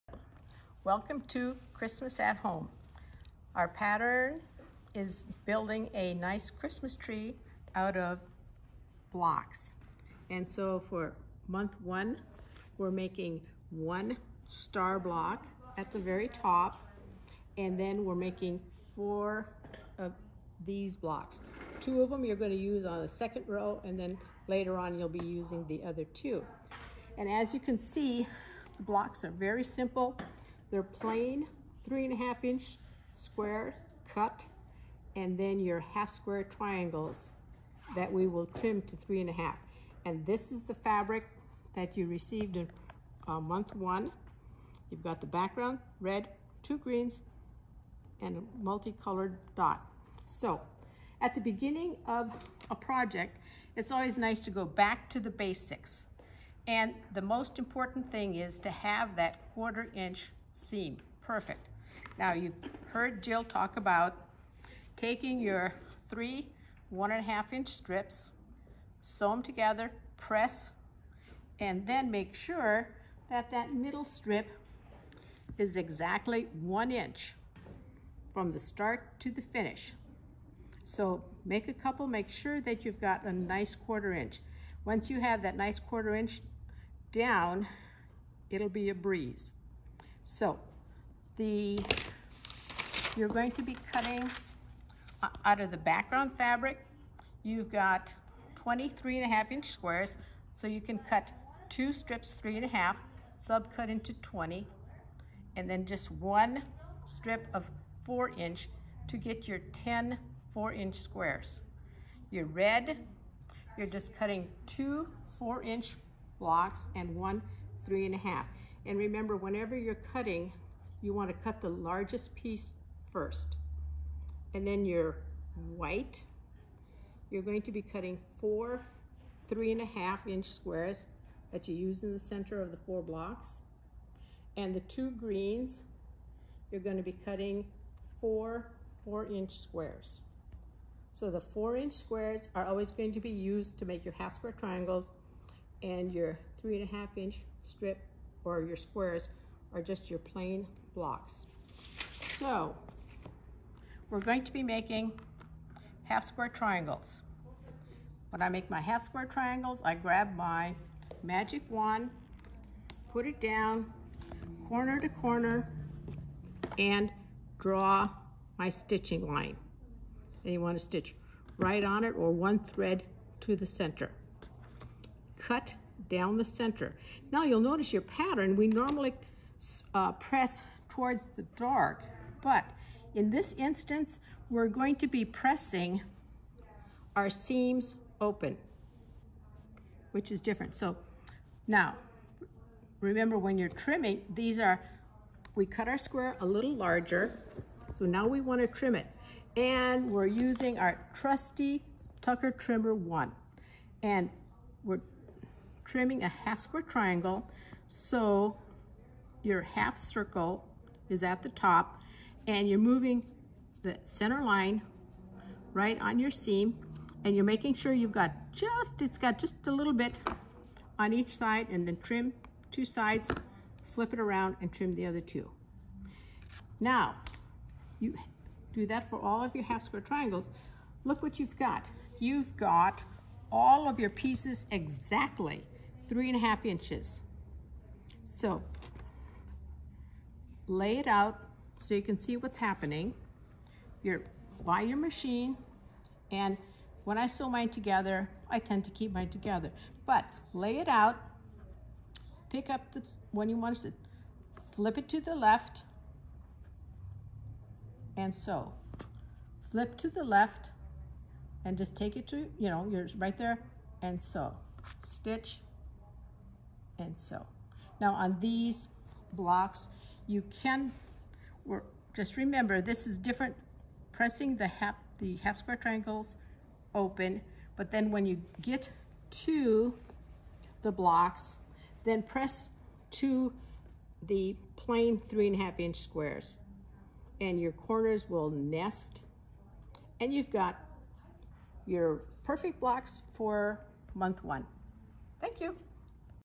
Christmas at Home BOM Month 1
Carolyn shares best techniques for making the blocks for month 1 of the Christmas at Home BOM. To see BOMs that are available to join